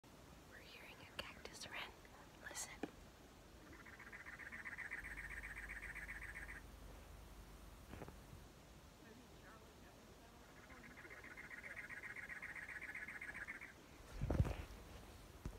Y'all Ever Heard A Cactus Wren!?🌵🐦🎶
Deep in the desert of Arizona we heard our first Cactus Wren and thought what thaaaaa ??
To survive the harsh climate these birds adapted to be creative hunters - they even know to check the front of cars for splattered insects as a nice meal!
Learn more about the Cactus Wren on our blog! ➡️  Mobilemushrooms.info ⬅️